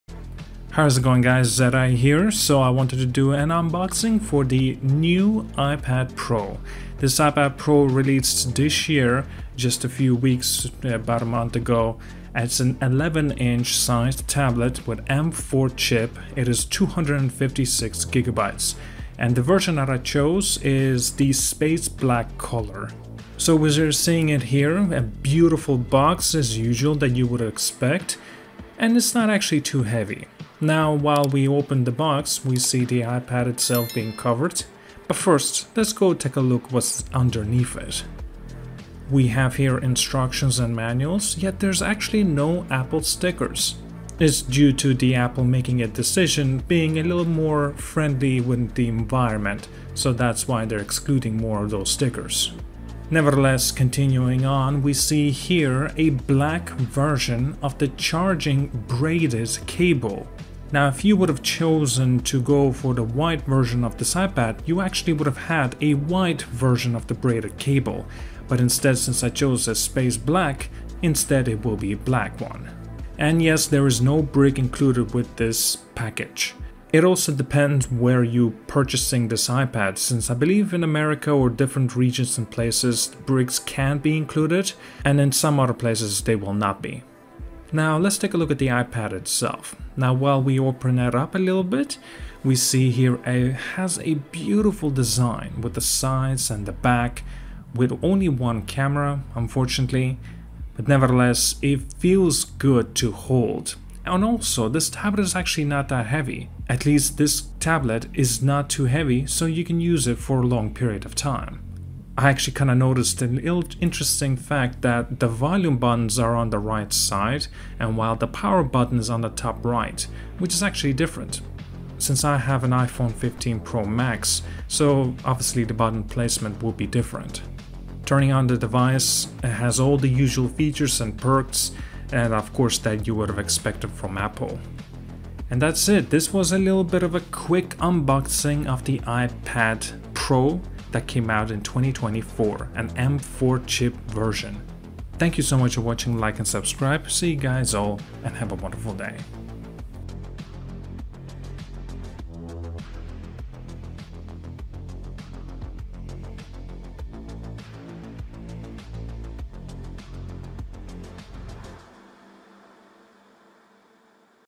New M4 iPad Pro (2024) Unboxing - My First Time with a Tablet!
Unboxing a new iPad Pro 2024 M4 version from the first-time perspective of having a new tablet...